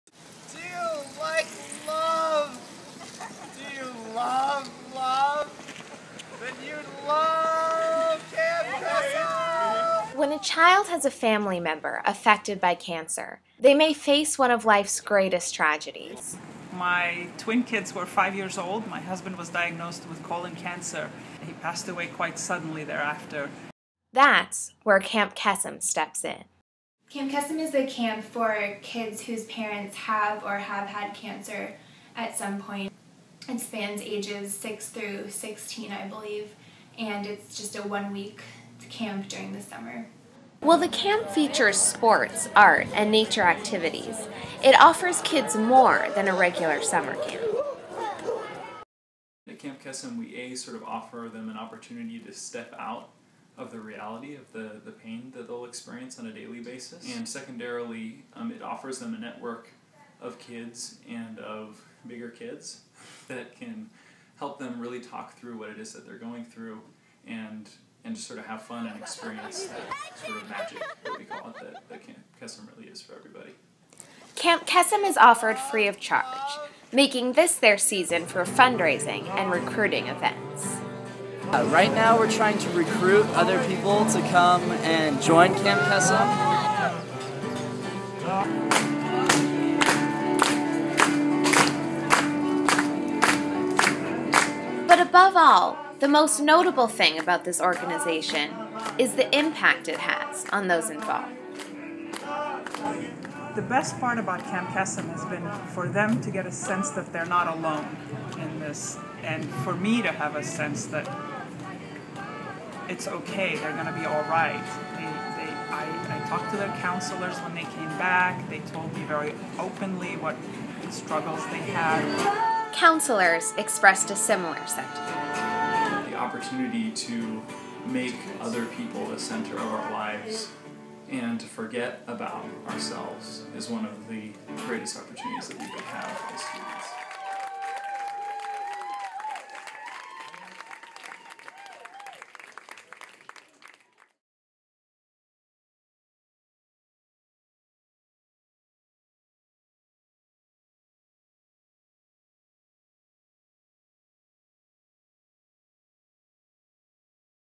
Students Host Camp for Children with Families Affected by Cancer
Camp Kesem, a summer camp for kids whose parents have or have had cancer, takes place on 37 university campuses across the United States as part of the larger organization, Camp Kesem National. Each year a group of students host Camp Kesem at Stanford, which takes place in the Santa Cruz Mountains in early June.

Camp Kesem at Stanford is offered free-of-charge. Organizers engage in year-round fundraising to put on the week of camp that allows children who've had an experience with cancer in their immediate families grow and bond over shared experience.